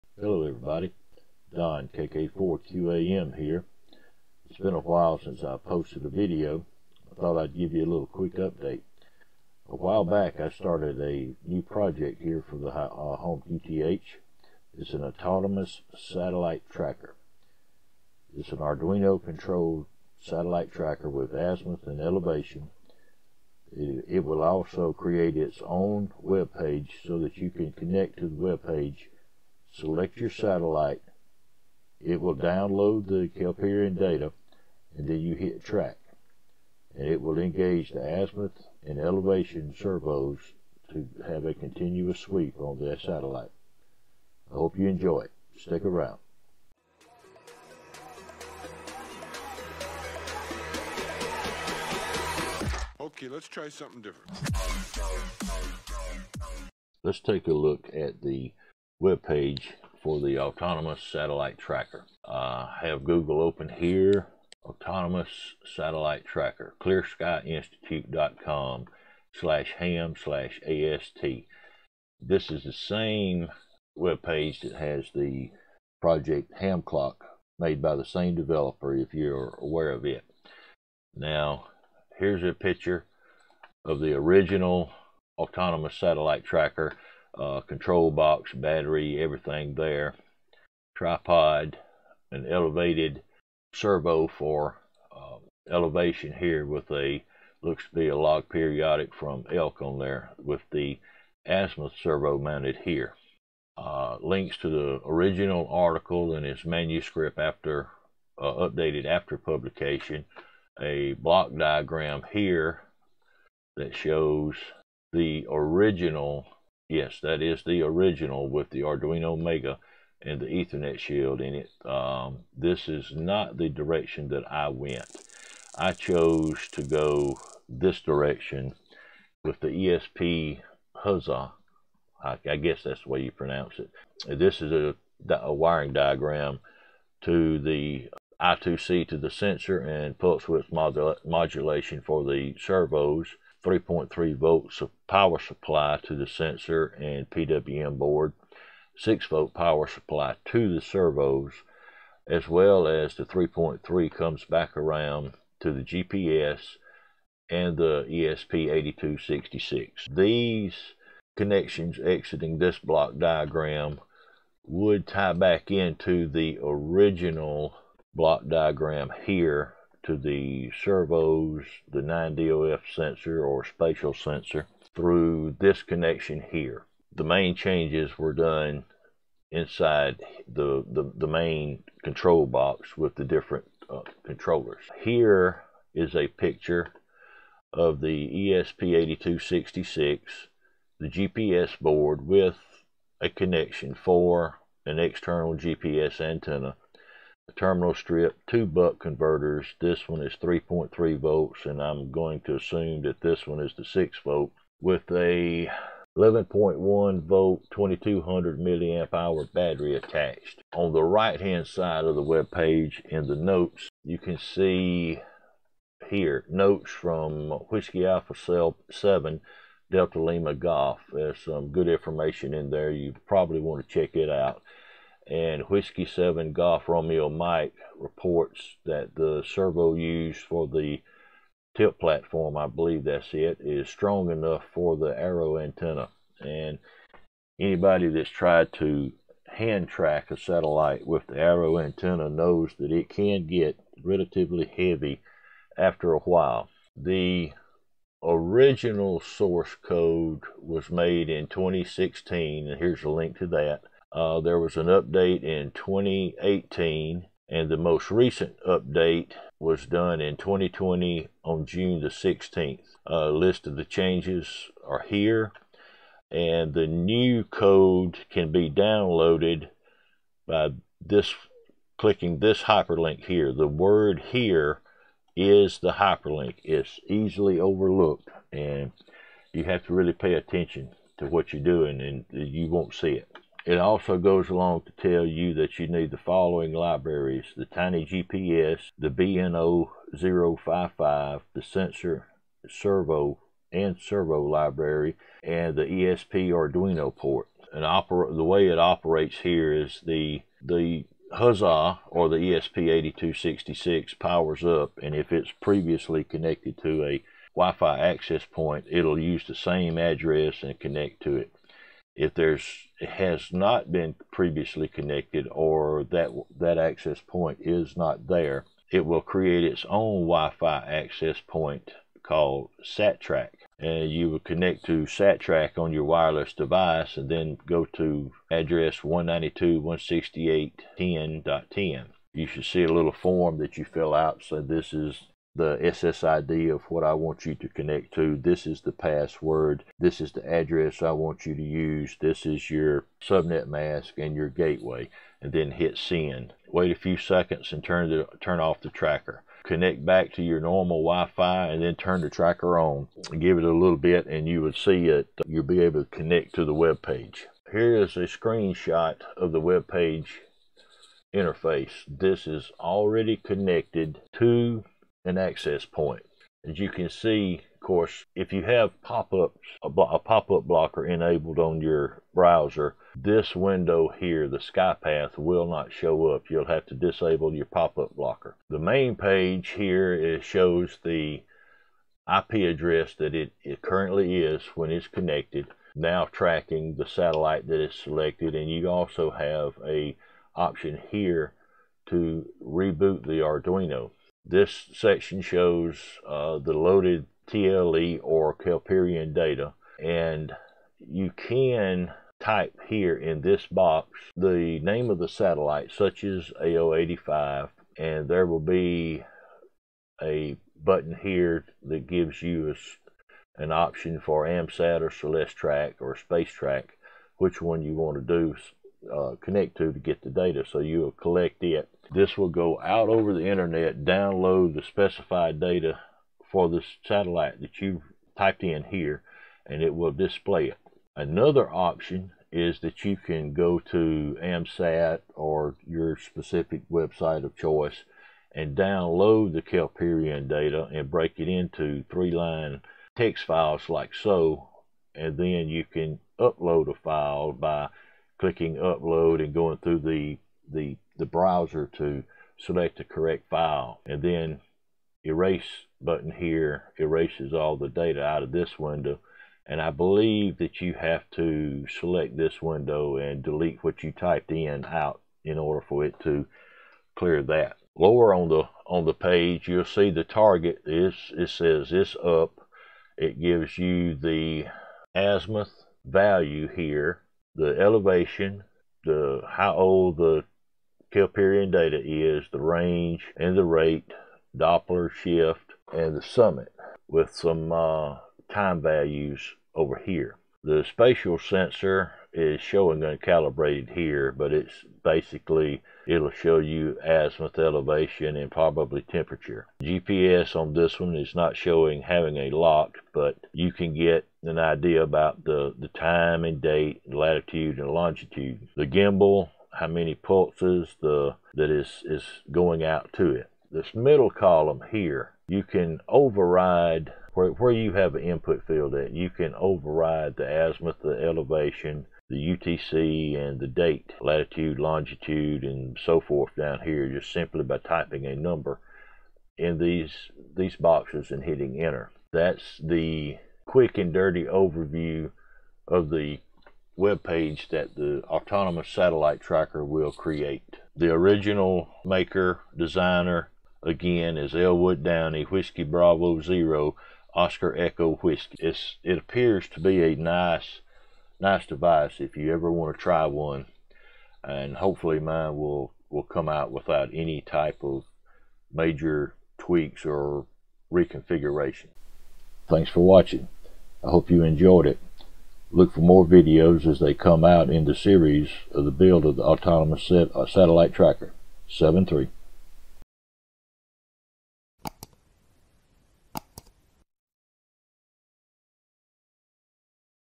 Autonomous Satellite Tracker Part 1 - Overview
We look at the original design by Elwood Downey, WB0OEW.